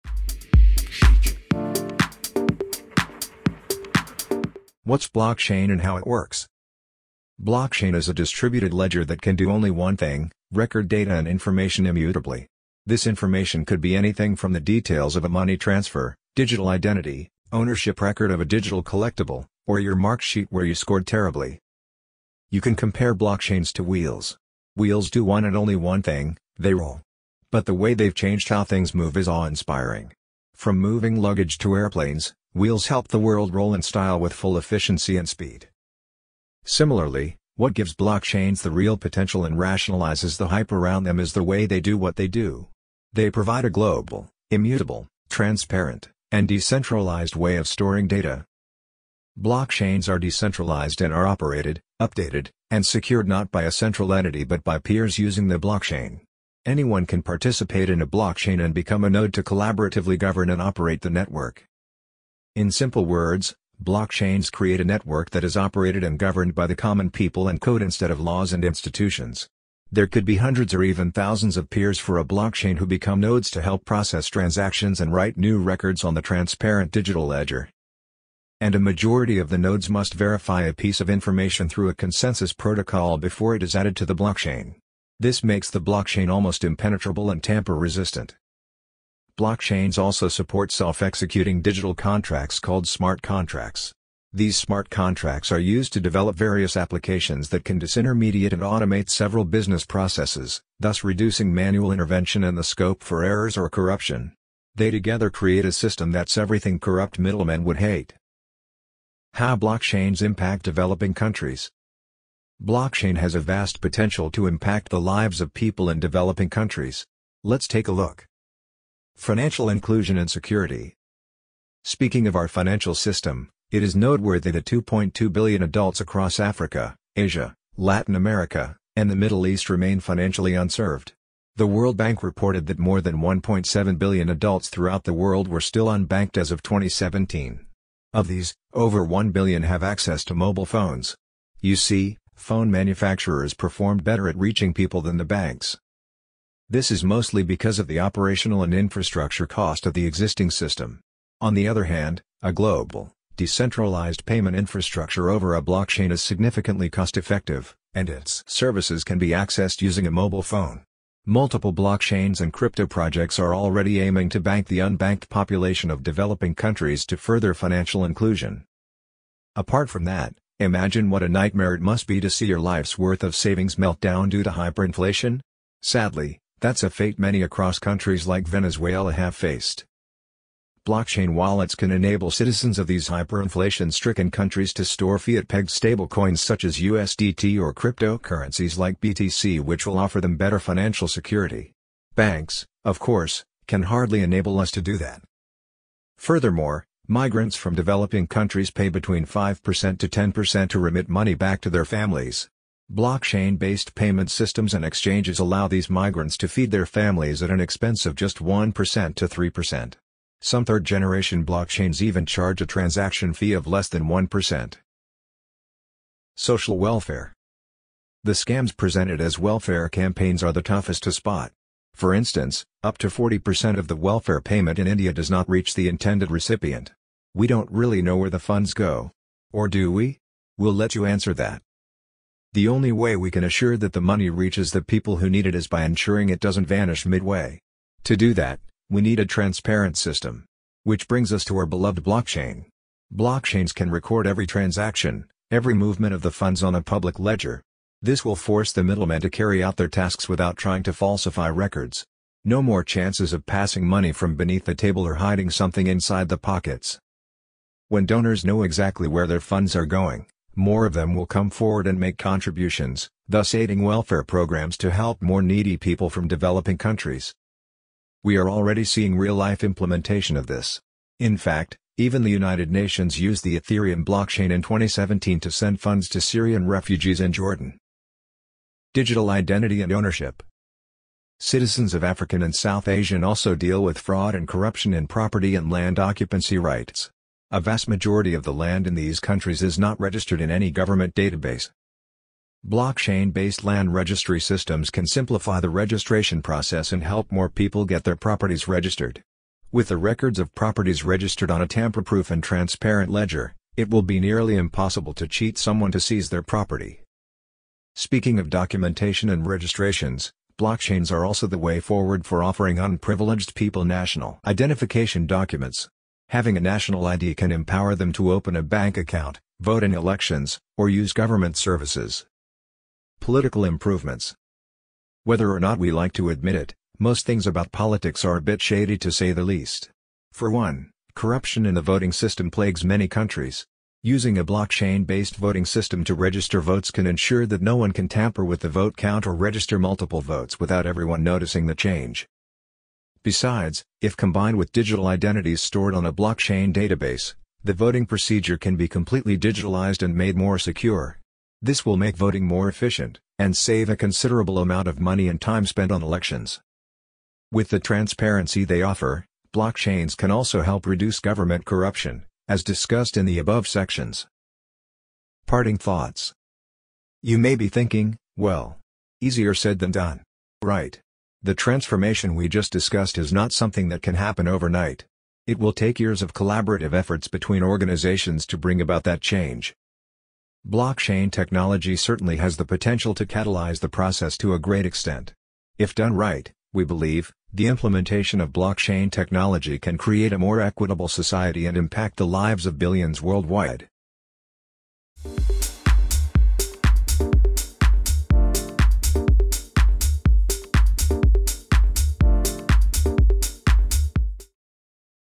Blockchain Revolution: Empowering Developing Countries
The Blockchain revolution is here and there’s a great chance it will empower developing countries. Let’s find out how.